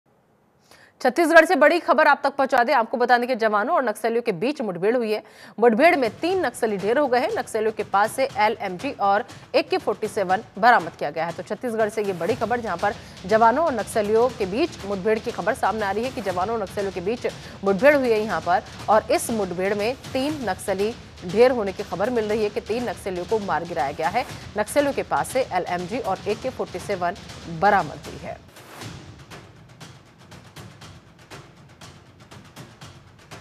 जवान औऱ नक्सलियों के बीच मुठभेड़ , इतने नक्सली ढेर || AK47 हुए बरामद || TRENDNG NEWS
About Video
जवान औऱ नक्सलियों के बीच मुठभेड़ , तीन नक्सली ढेर || AK47 हुए बरामद ||